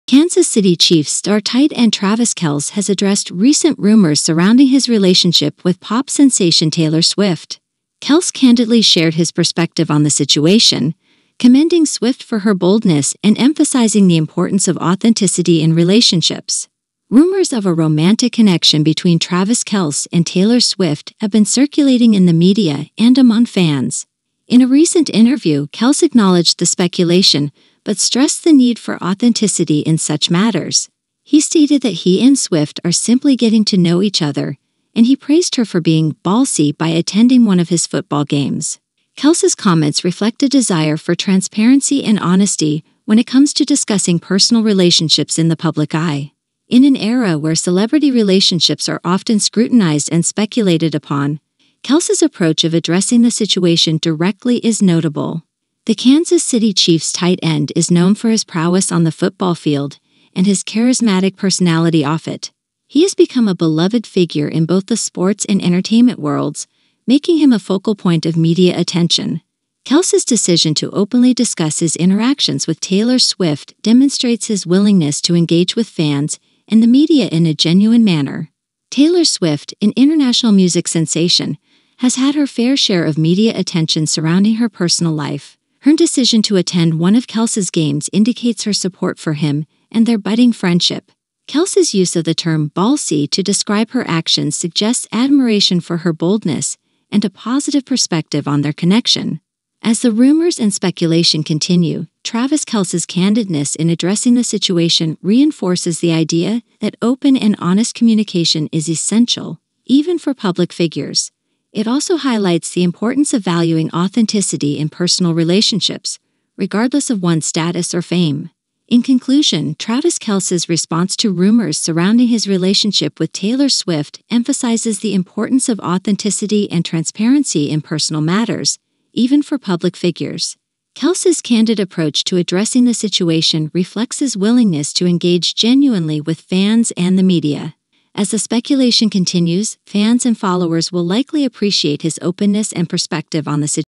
Travis Kelce dishes on 'what's real' amid Taylor Swift rumors, calls her 'ballsy' for showing up to
NFL star Travis Kelce opens up about the swirling rumors surrounding his connection with Taylor Swift, shedding light on the authenticity of their relationship. He commends Swift's boldness for showing up to support him. Join us as we delve into Kelce's perspective on this high-profile bond and the public's fascination with their romance.